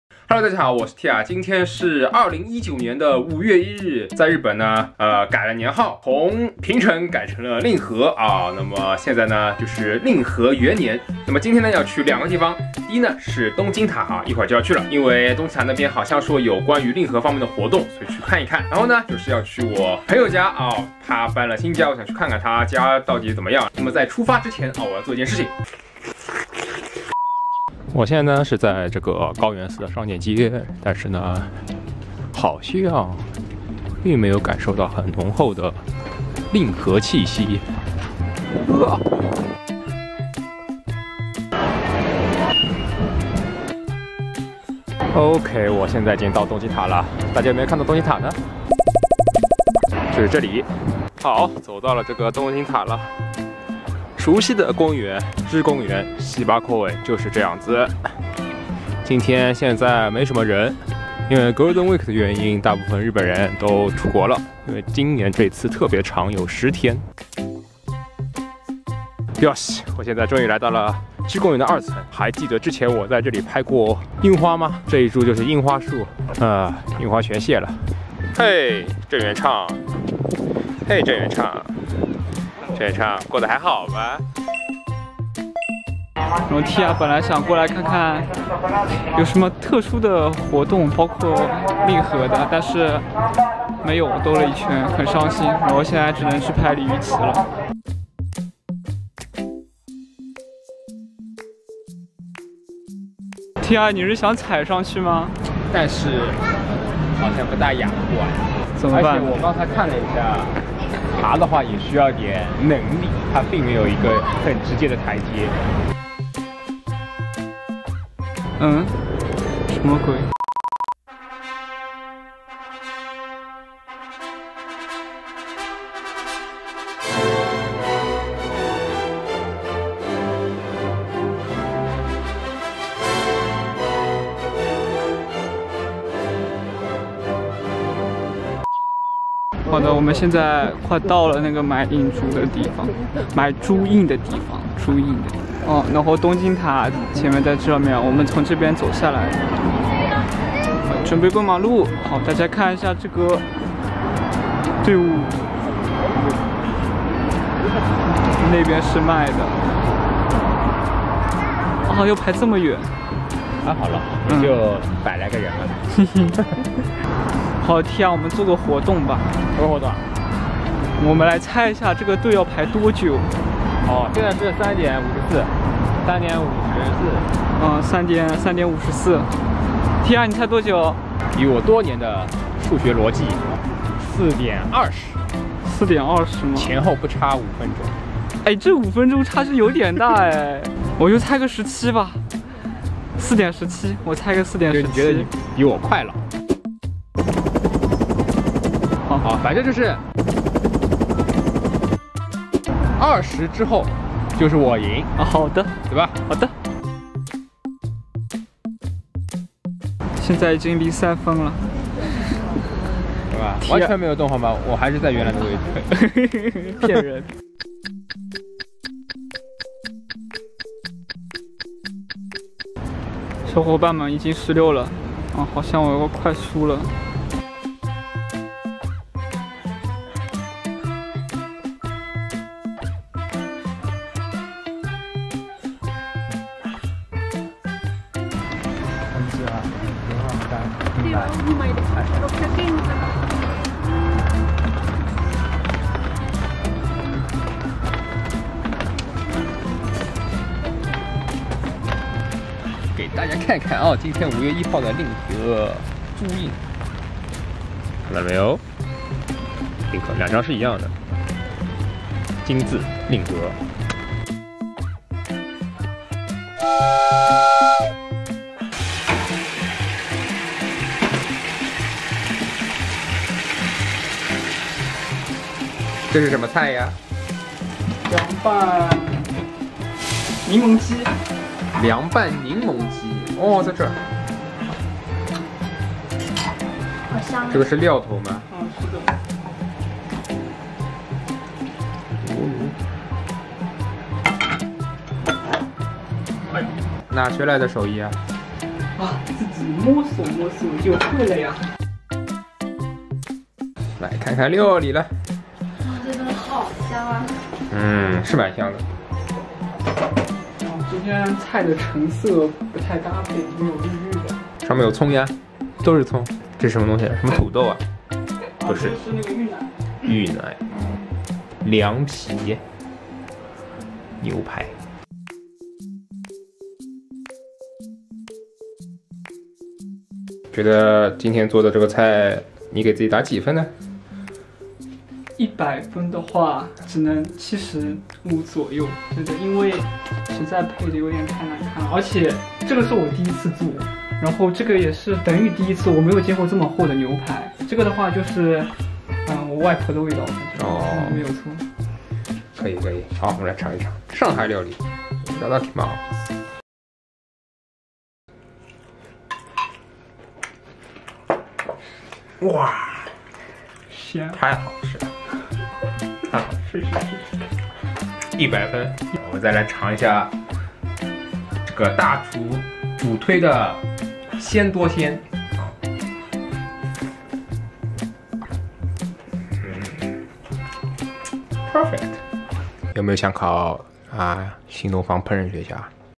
【Tiya的日本留学日记】vlog172 令和第一天去东京塔买朱印&品尝20岁天才少年做的饭
令和元年快乐！
五一放假快乐！
哦，传完视频假期已经结束了